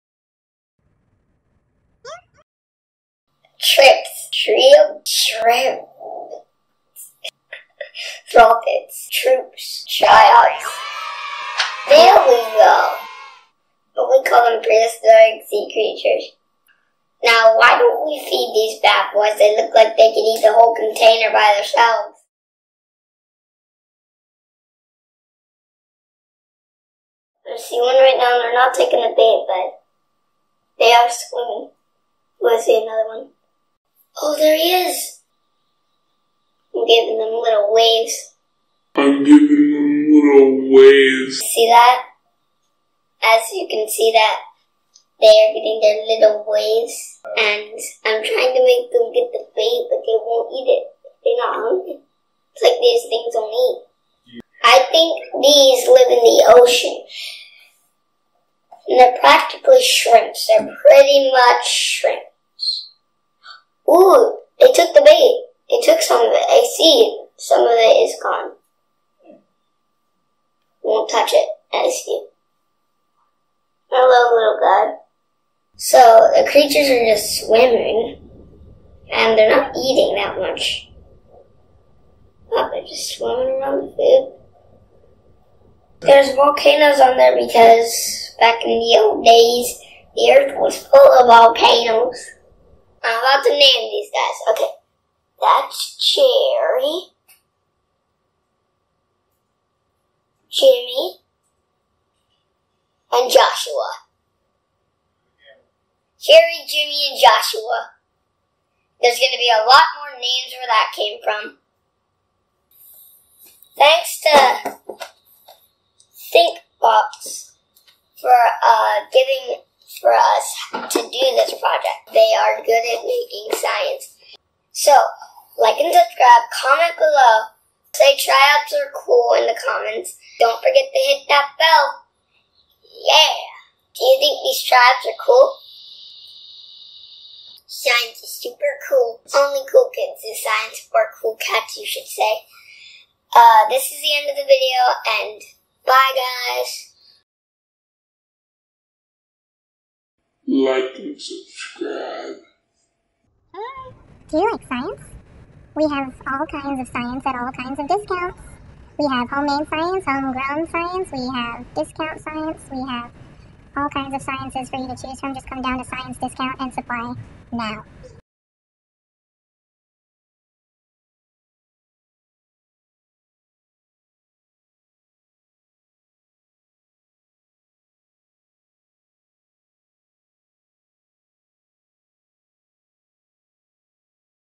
Prehistoric Sea Creature Terrarium PART 2 | Science Time
Join LPK on a magical journey through time while we UNBOX a Prehistoric Sea Creature Terrarium! This is the first video in our terrarium adventure. First we unbox and set up the terrarium with the TRIOPS eggs (prehistoric sea creatures). In the next video we see how big they grow! It took just 3 days for the Triops to start hatching, now they are growing!